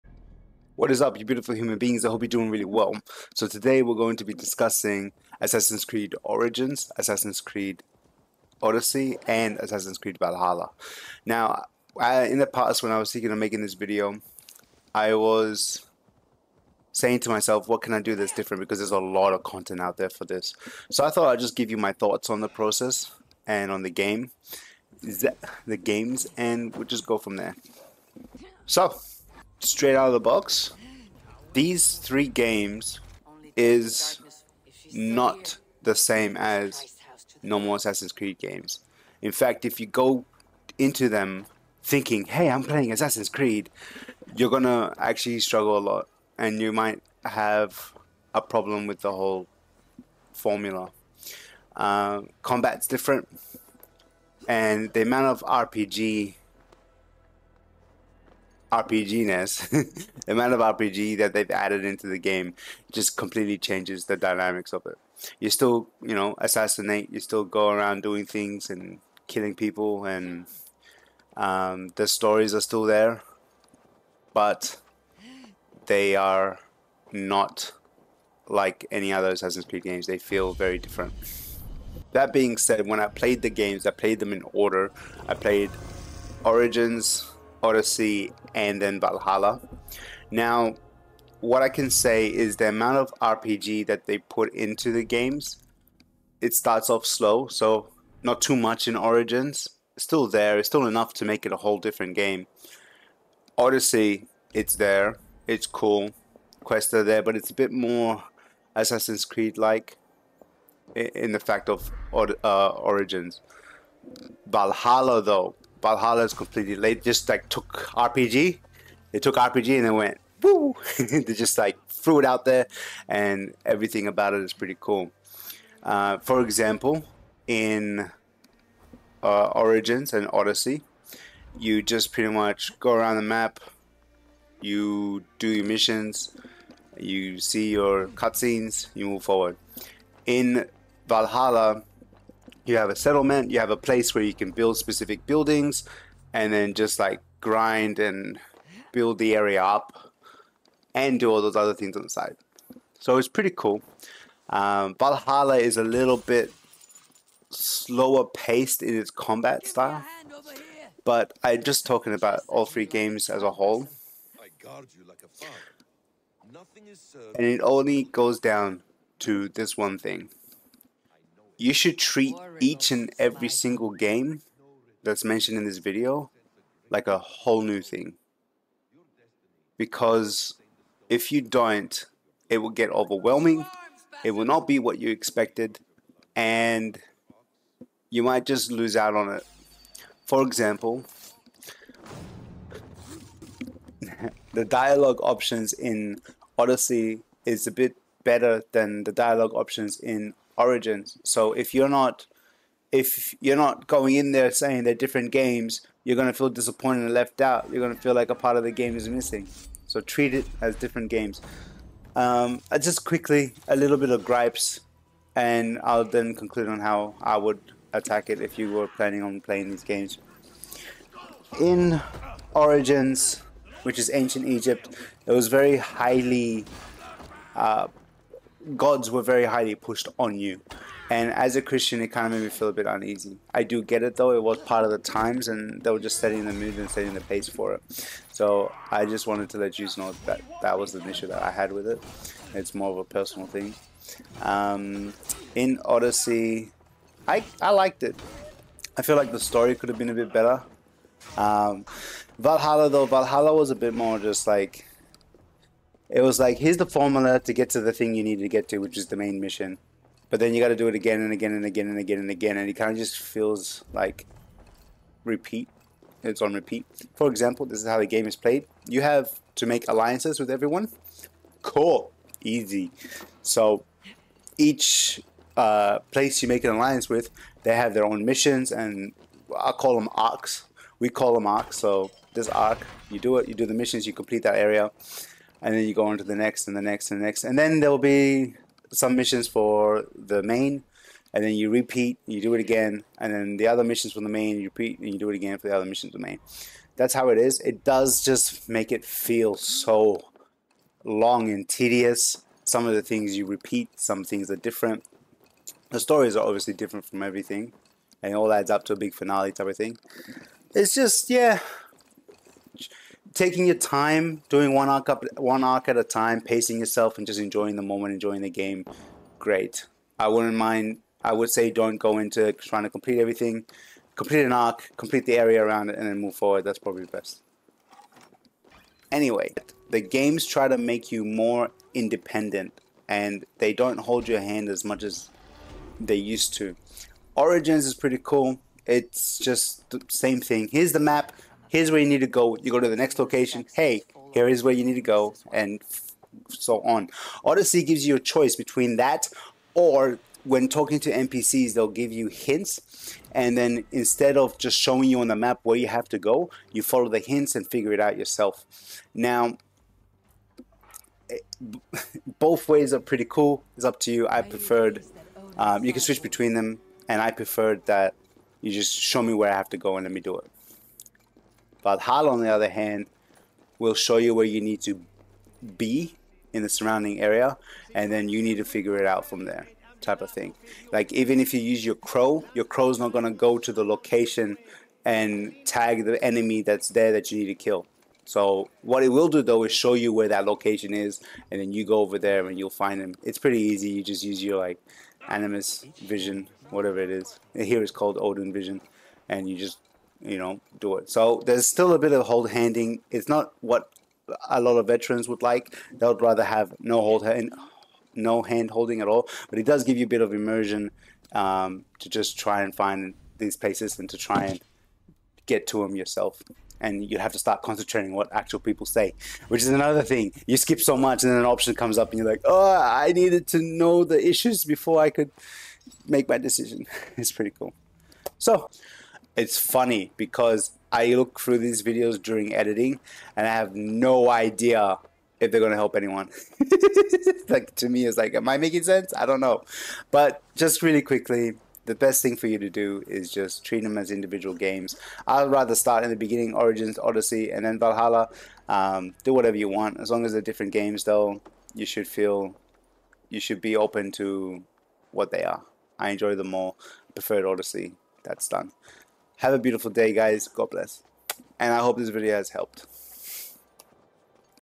First Time Playing? Overwhelmed on Starting? Assassins Creed Tips and review
when i first wanted to play these games i was a little lost on where to start and what to expect in this short video i try my best to give some tips  on how to tackle these three games,
feedback is always welcomed take care